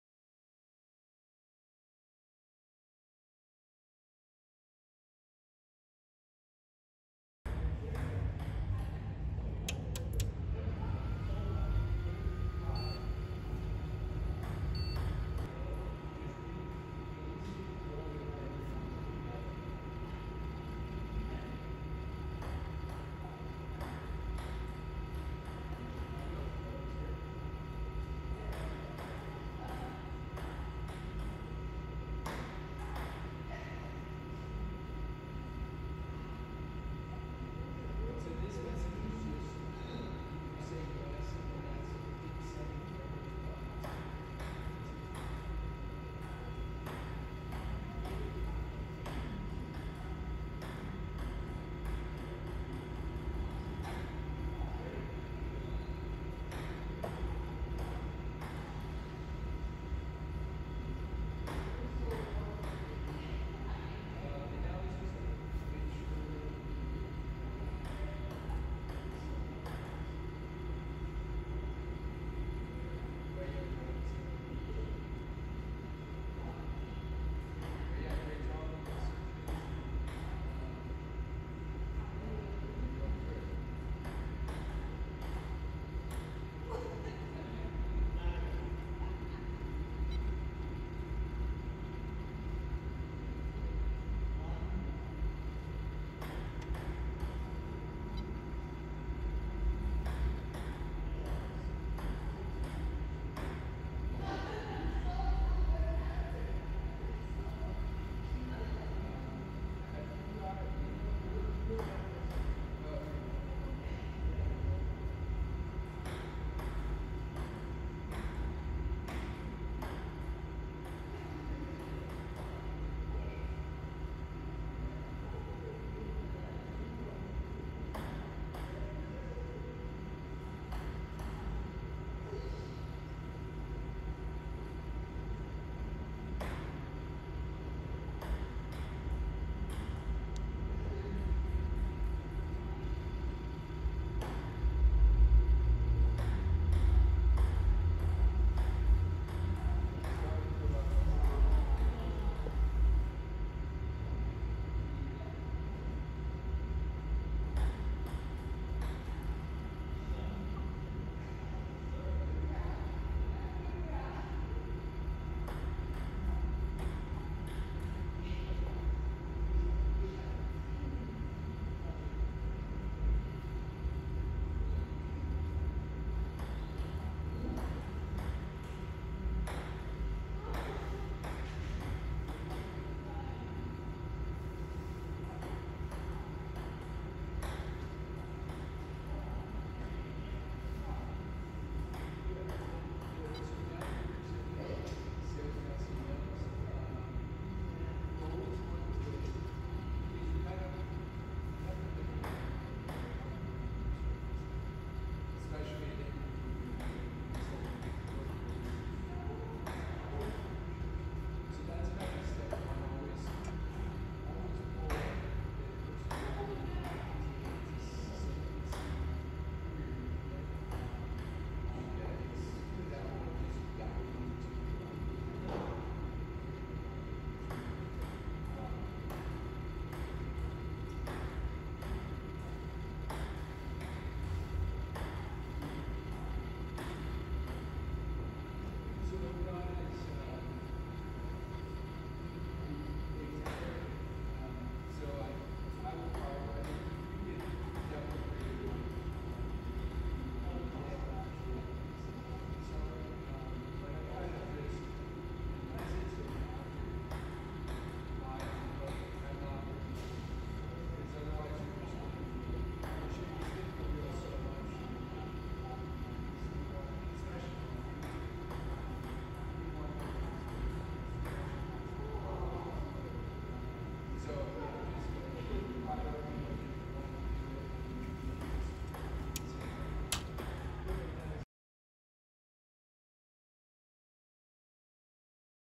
Agilent 8163 A Lightwave Multimeter (A# 65316)
Bridge Tronic Global, Inc has this Agilent 8163 A Lightwave Multimeter (A#65316) available for sale at our facility in Fountain Valley, California.